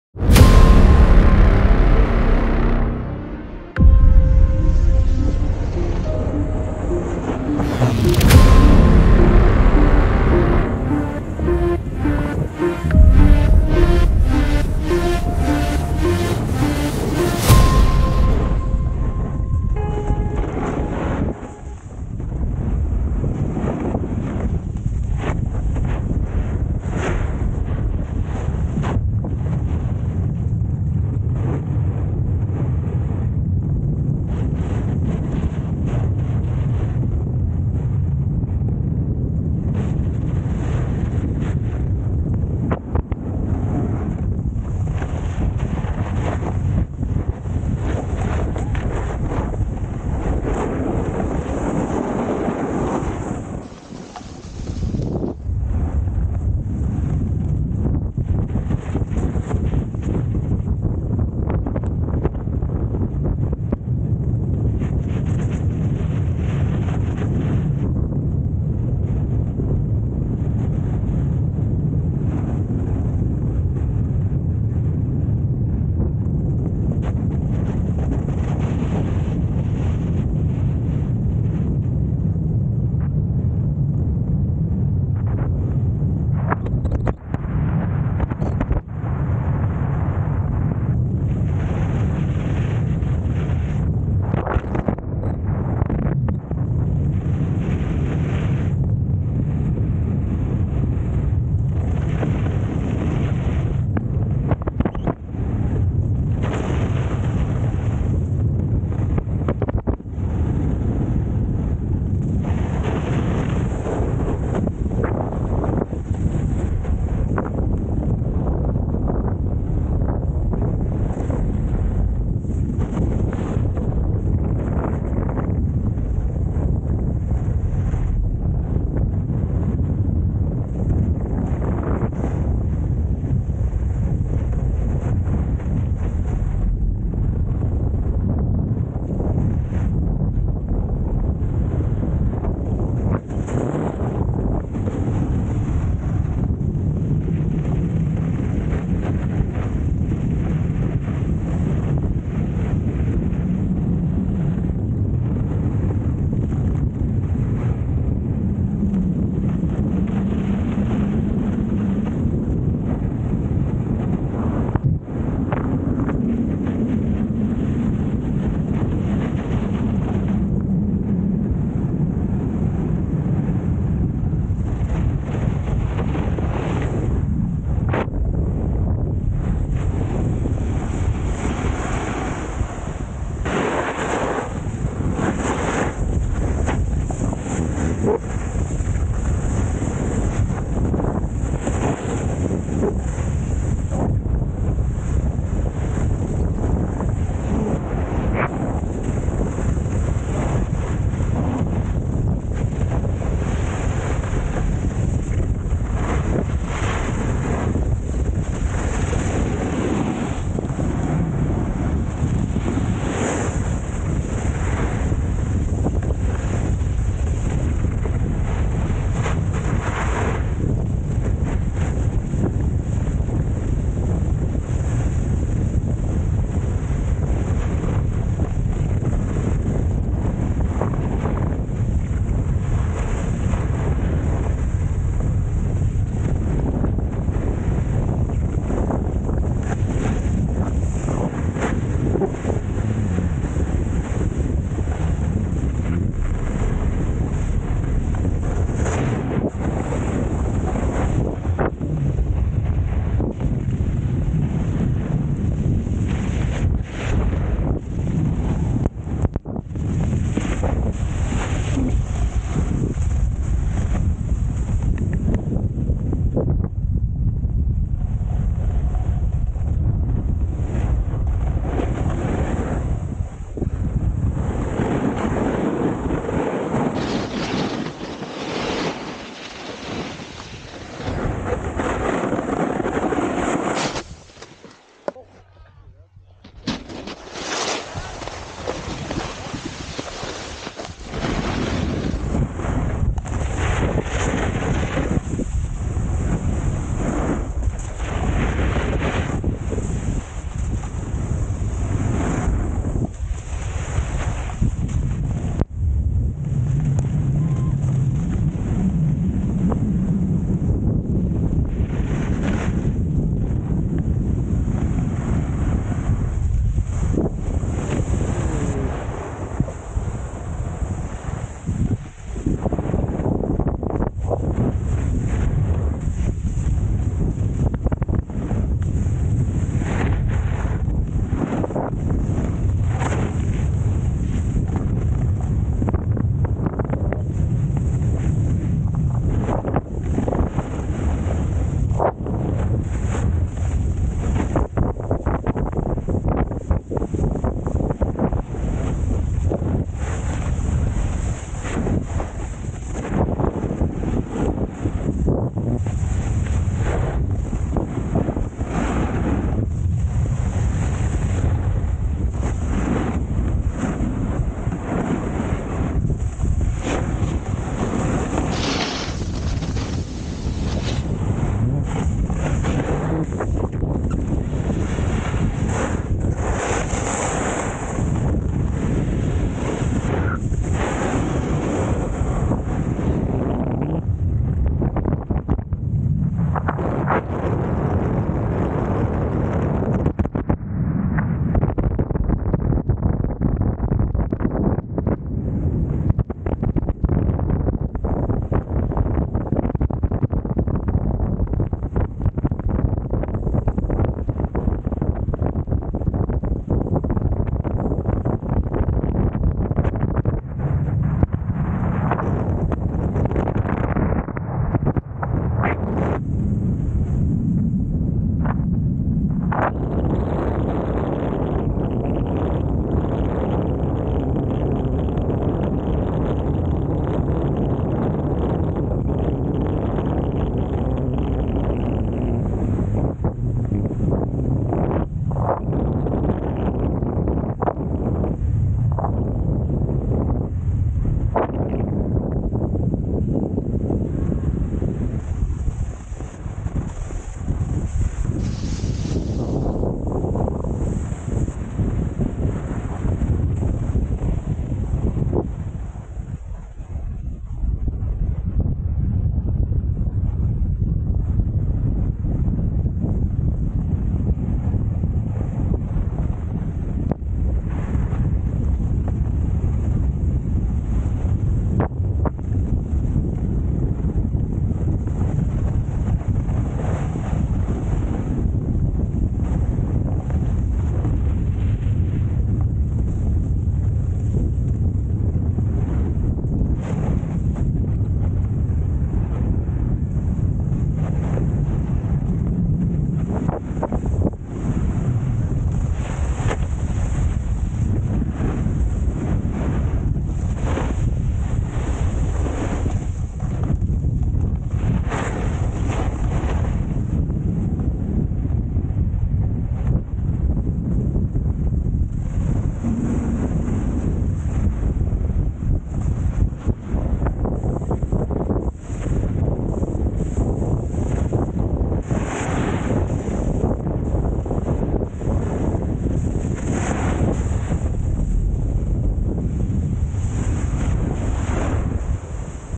Full length (uncut) carving from Nagens to Flims (Laax)
The run start at Nagens at 2127 amsl, follows the slope 12, 18 and 10, ends at Flims 1100 amsl